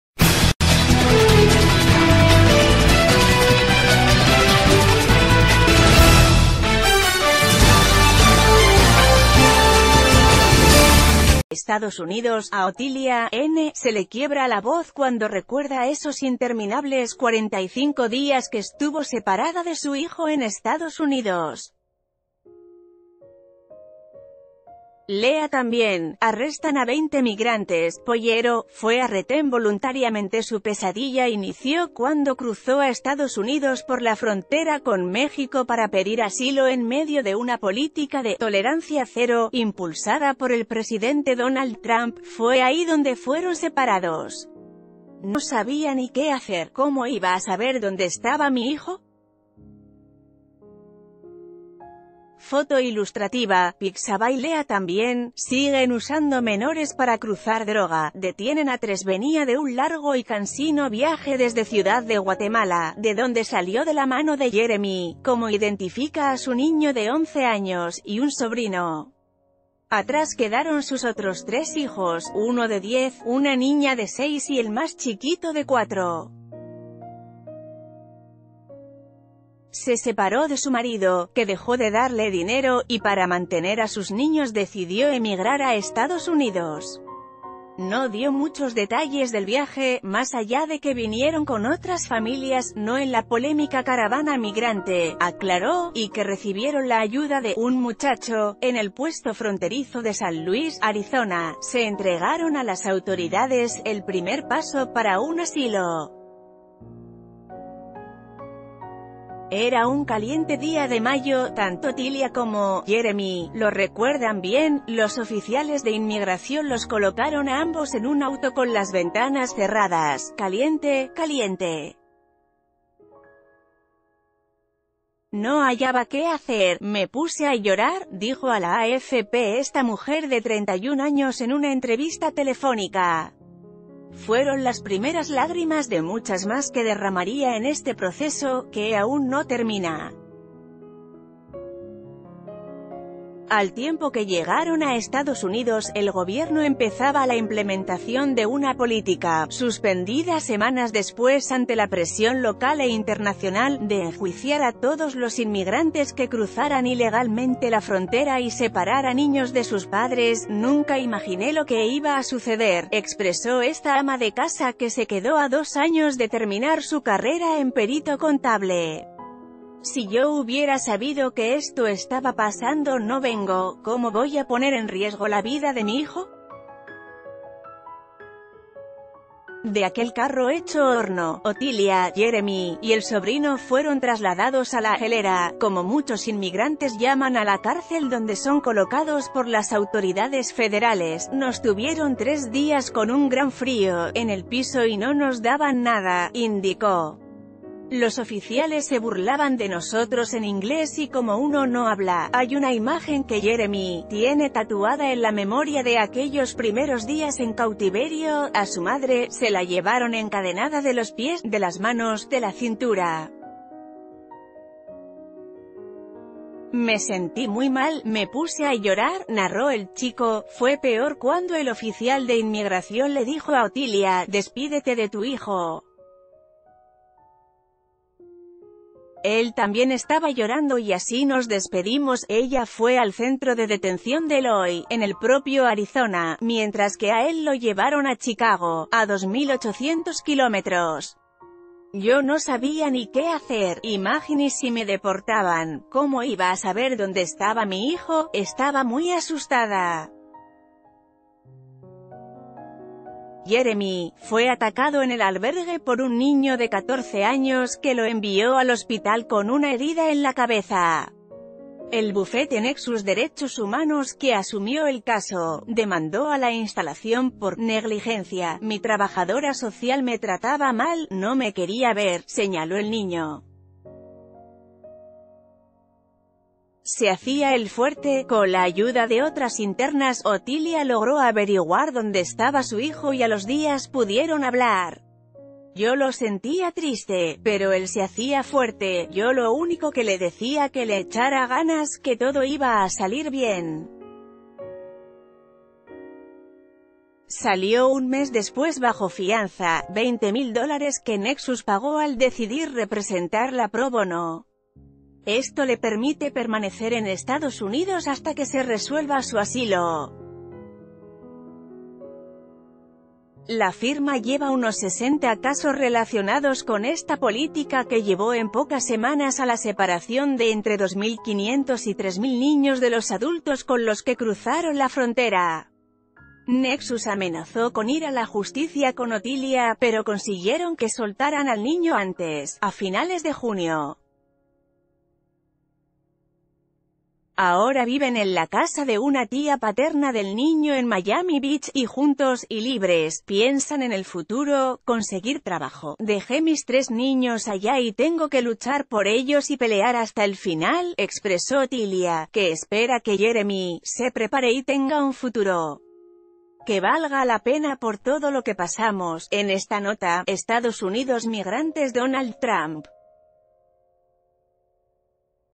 "No sabía ni qué hacer, ¿cómo iba a saber dónde estaba mi hijo?"
Estados Unidos.- A Otilia "N" se le quiebra la voz cuando recuerda esos interminables 45 días que estuvo separada de su hijo en Estados Unidos. Lea también: Arrestan a 20 migrantes; "Pollero" fue a retén voluntariamenteSu pesadilla inició cuando cruzó a Estados Unidos por la frontera con México para...